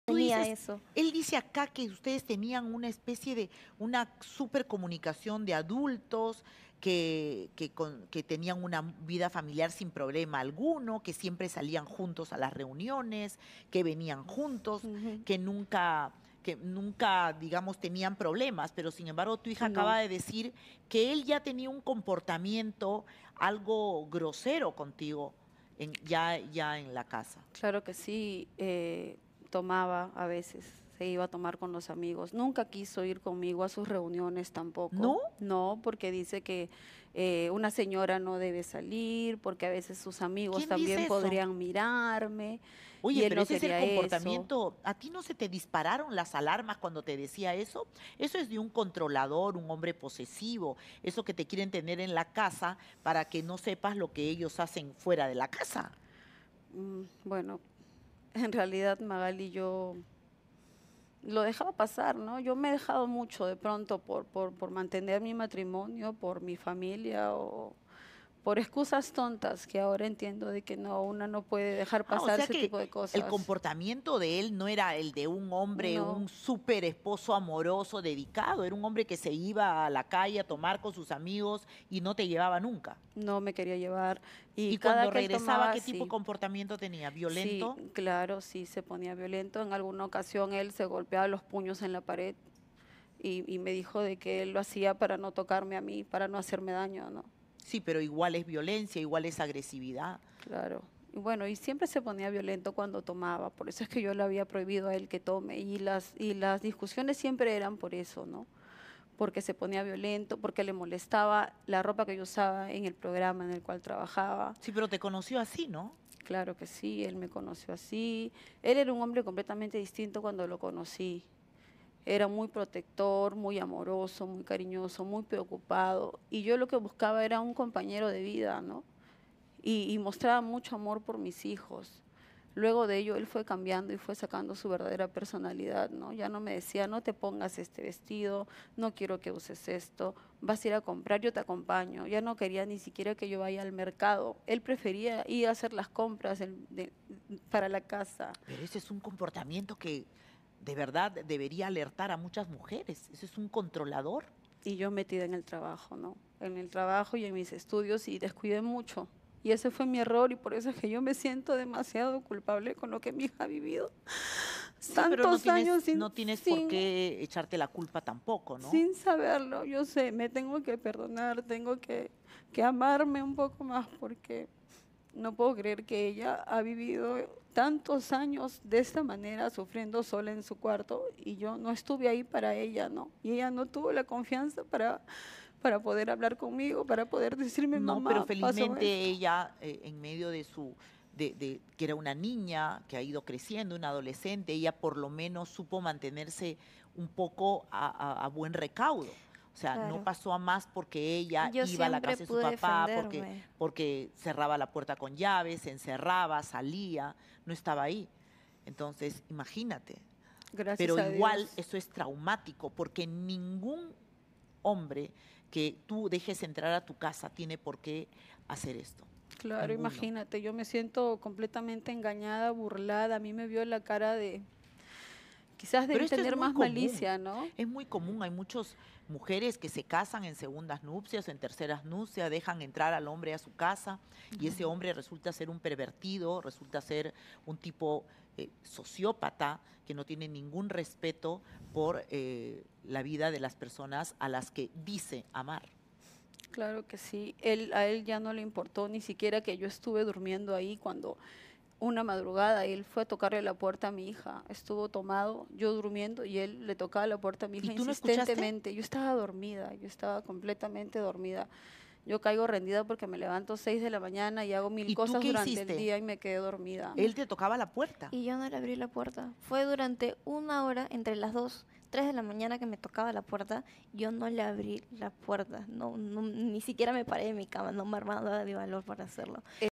Leslie Moscoso se quiebra por su hija: "Me siento demasiado culpable, no estuve ahí para ella"
La actriz cómica no se perdonó el hecho de haber abandonado a Mayle.